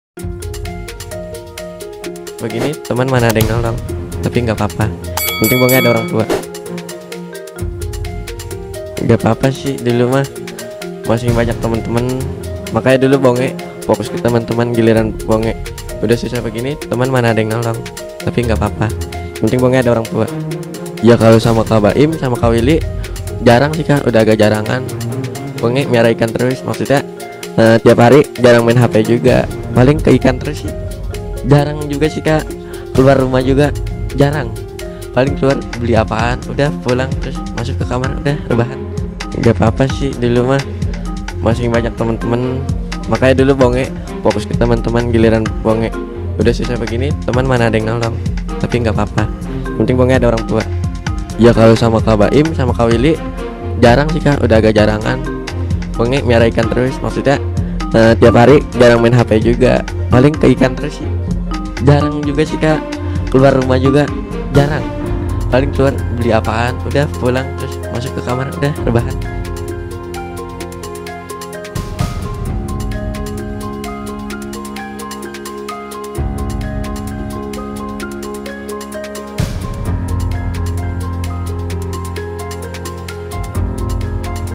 Trend Citayam Fashion Week Mulai Meredup Begitu Dengan Bonge
Trend Citayam Fashion Week telah meredup begitu pula dengan sosok Bonge. Dulu ia selalu menjadi pusat perhatian dan sering diundang tampil di acara televisi hingga melakoni menjadi model fashion show.

Kini popularitas Bonge pun perlahan mulai redup. Wajah Bonge kini nyaris tidak pernah muncul di berbagai acara televisi.

Pria yang dulunya harus mengatur waktu karena padatnya jadwal undangan kini lebih banyak menghabiskan waktu di rumah mengurus ikan.

Selain itu Bonge juga kini merasa ditinggalkan oleh teman-temannya.

"Dulu Bonge fokus ke teman-teman, udah susah begini teman mana ada yang nolong", kata Bonge lebih lanjut.

"Tapi nggak apa-apa yang penting Bonge ada orangtua", ujar Bonge.

ARP